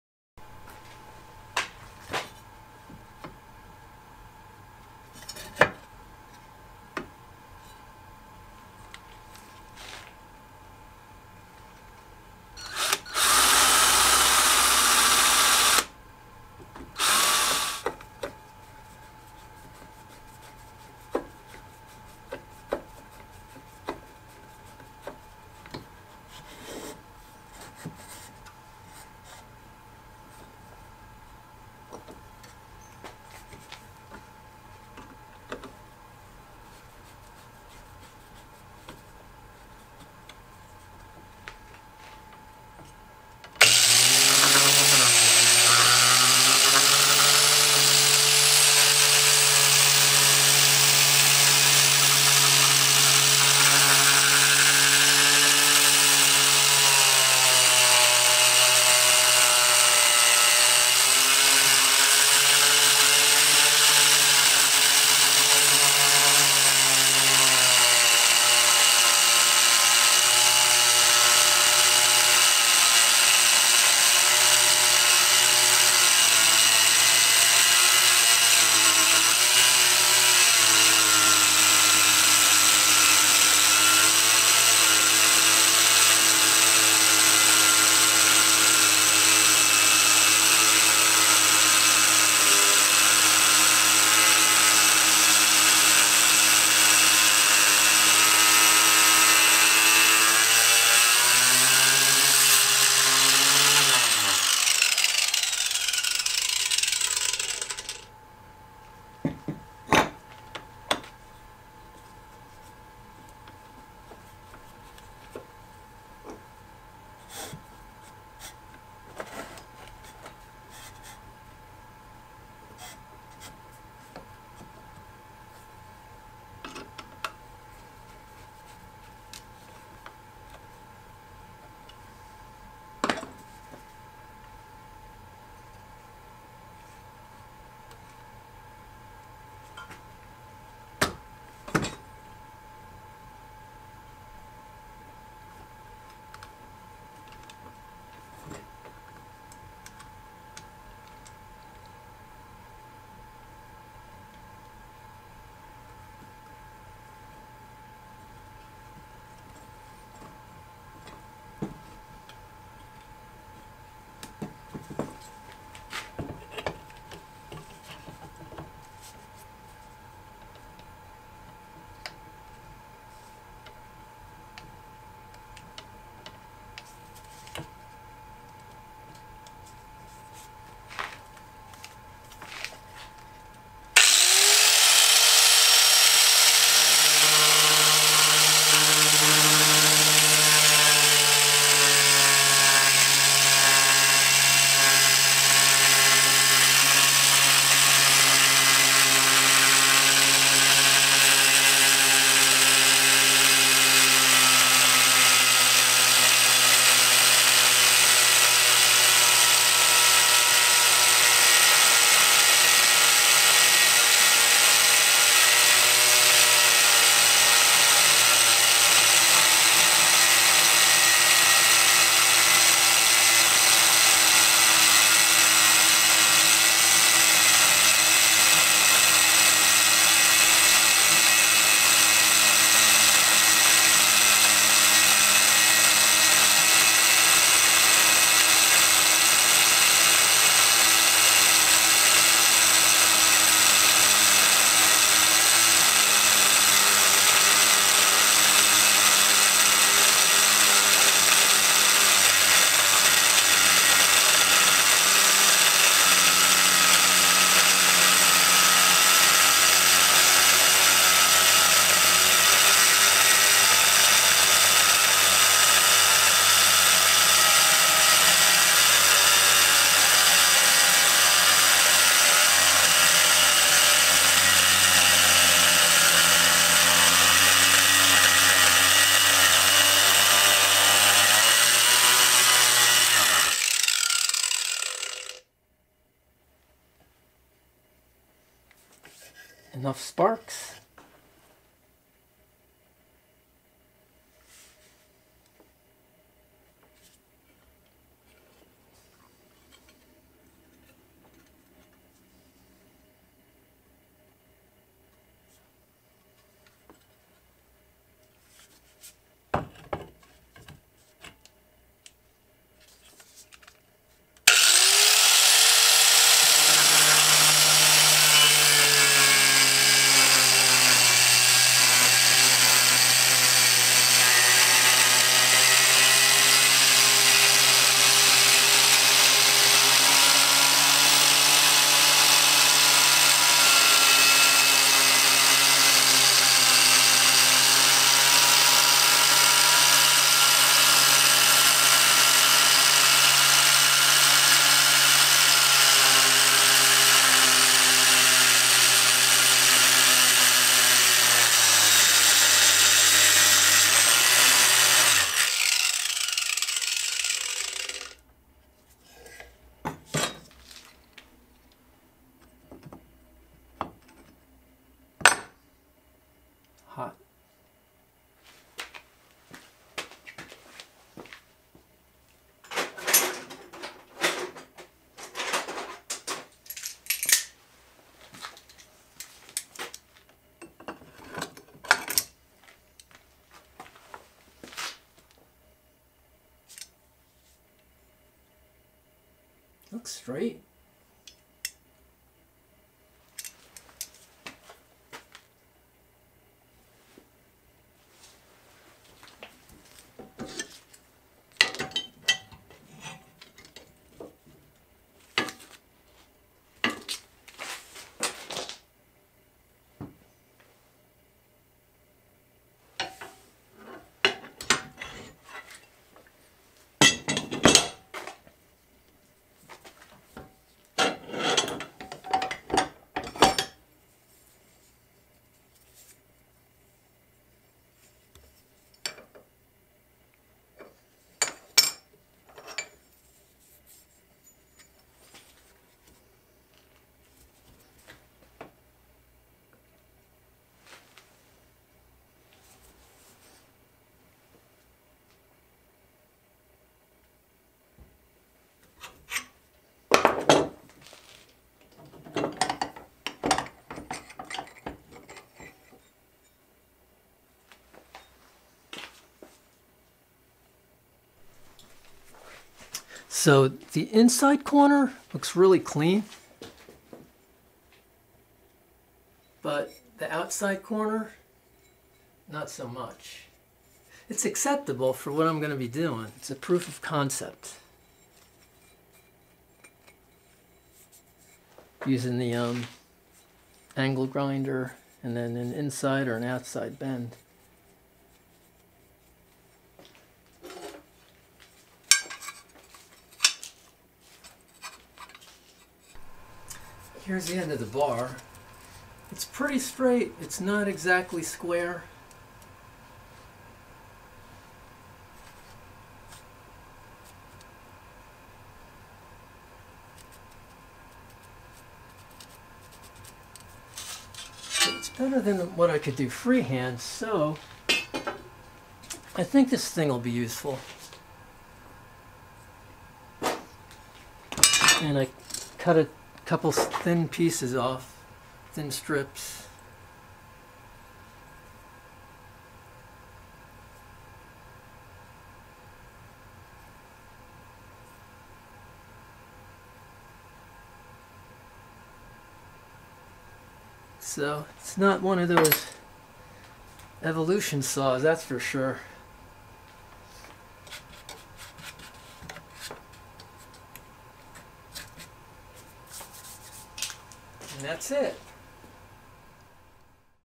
Using the Harbor Freight Angle Grinder Guide
The Harbor Freight guide for their angle grinder worked pretty good.